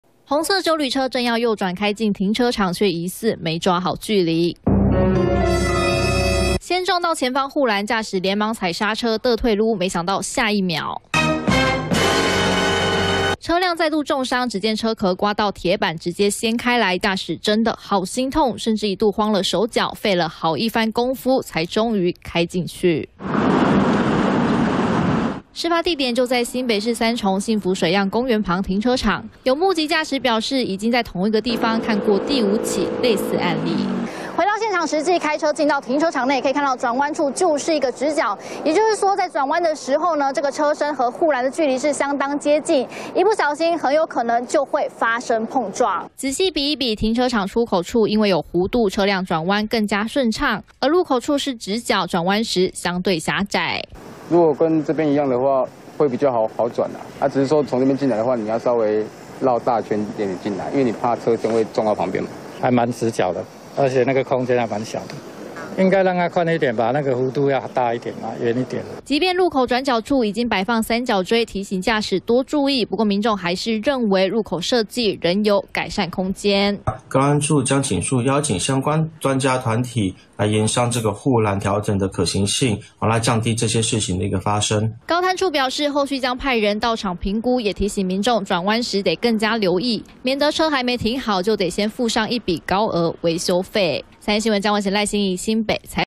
好痛！停車場入口「直角殺」 車轉彎慘遭刮傷│94看新聞
友邦連結區
◎ 訂閱【三立新聞網】最新影片搶先看